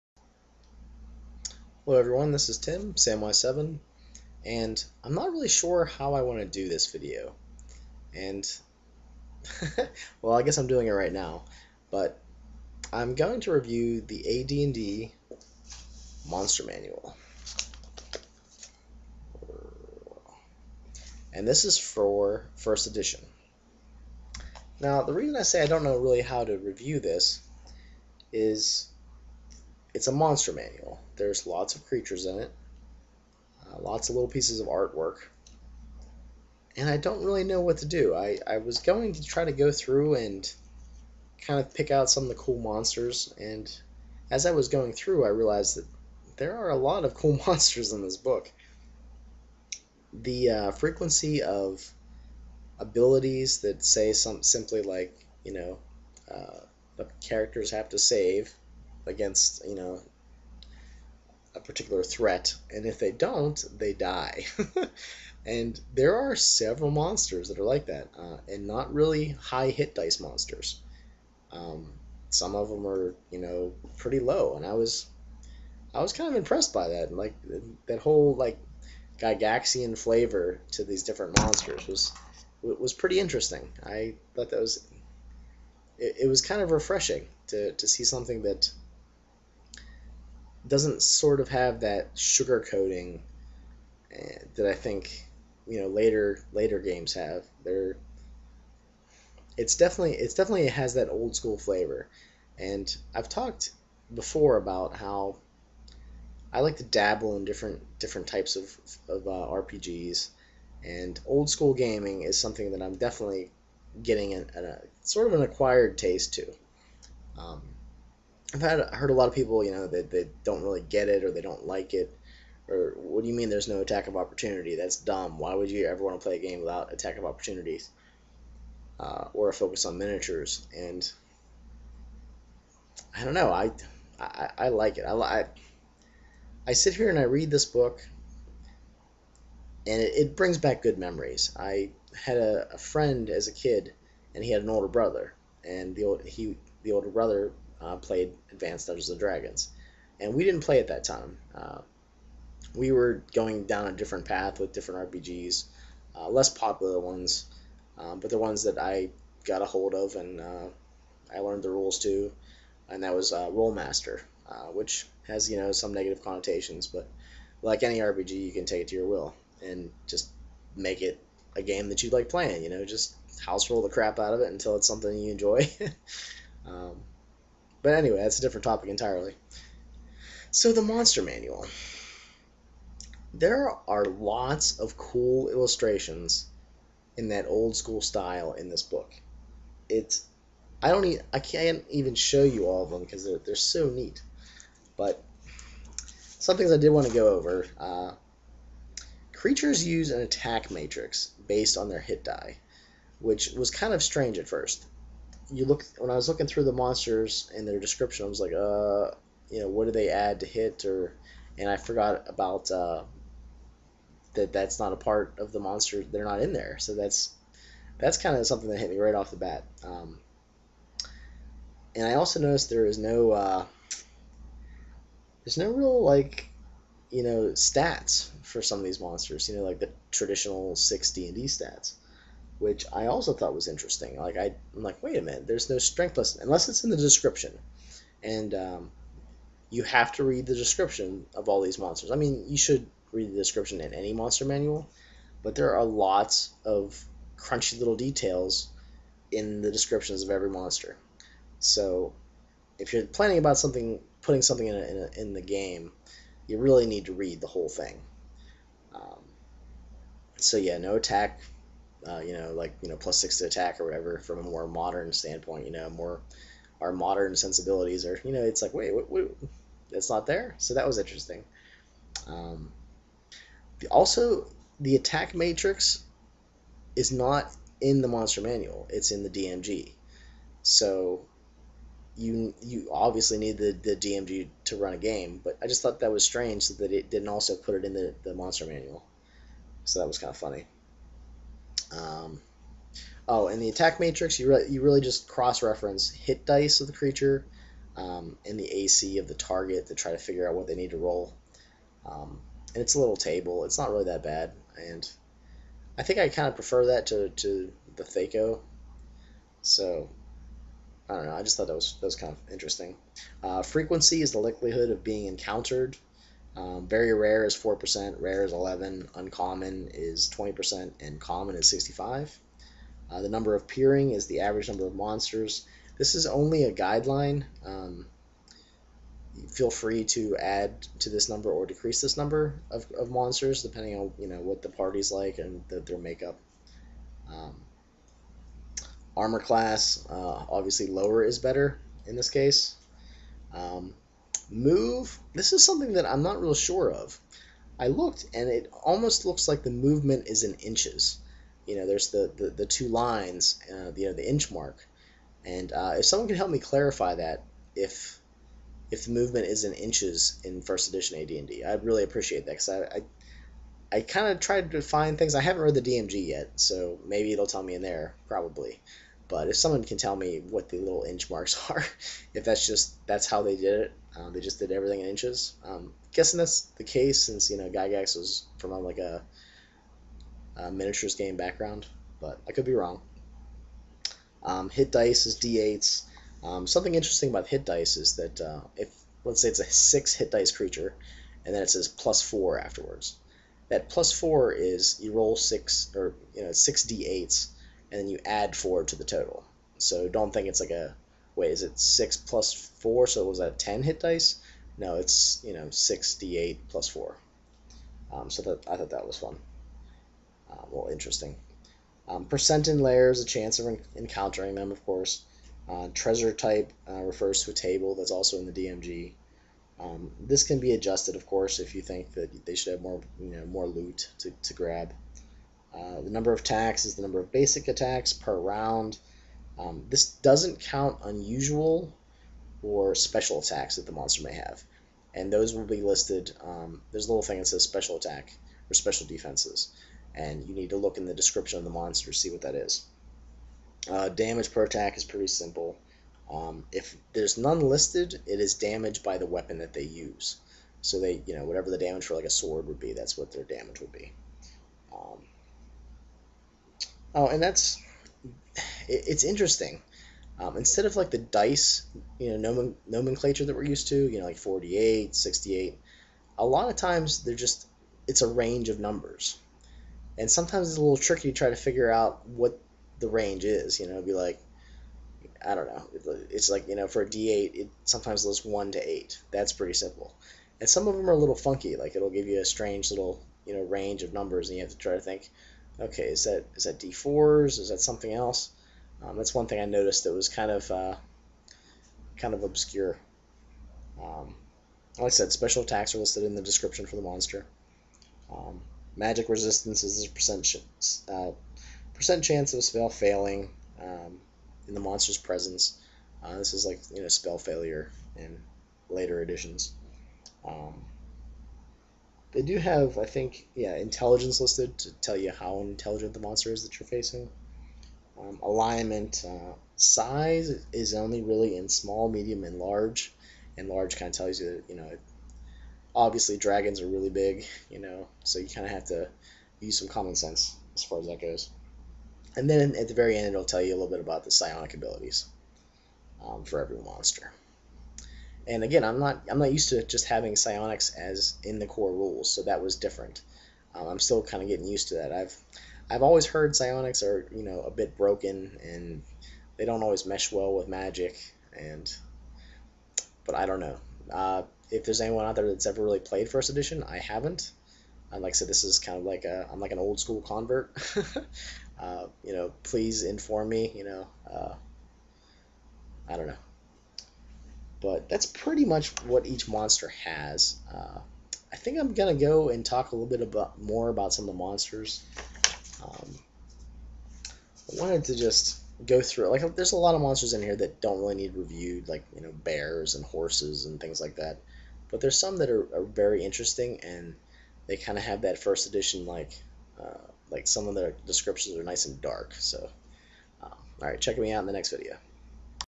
AD&D 1st Edition Monster Manual RPG Review Part 1
I wax poetic about the AD&D 1st Edition Monster Manual.  Feel the Old School Goodness.